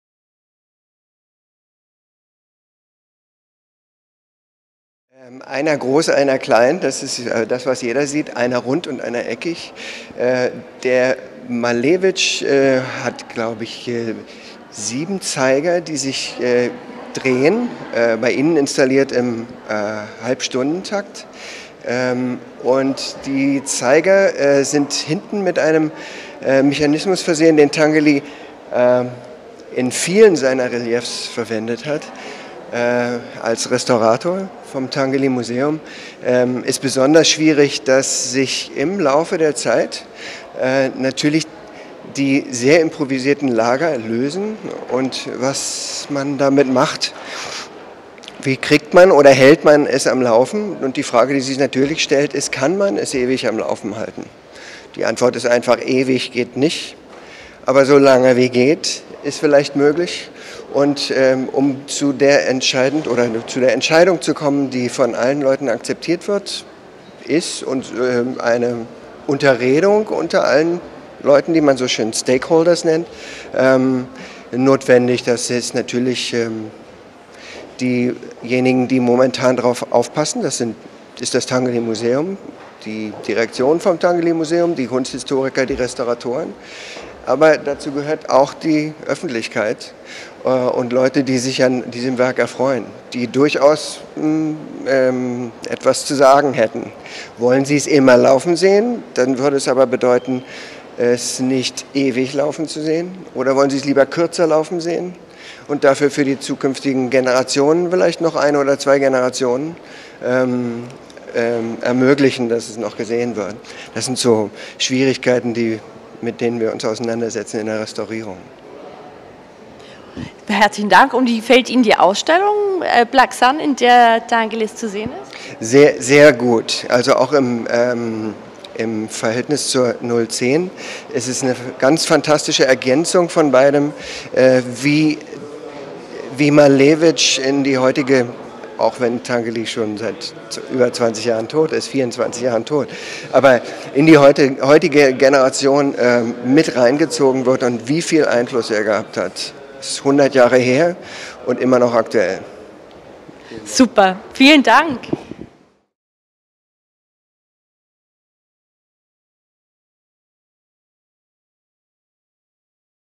Auf der Suche nach 0,10: Interview mit Albrecht Gumlich
Auf der Suche nach 0,10 – Die letzte futuristische Ausstellung der Malerei: Interview mit Albrecht Gumlich, Restaurierung und Konservierung, Museum Tinguely.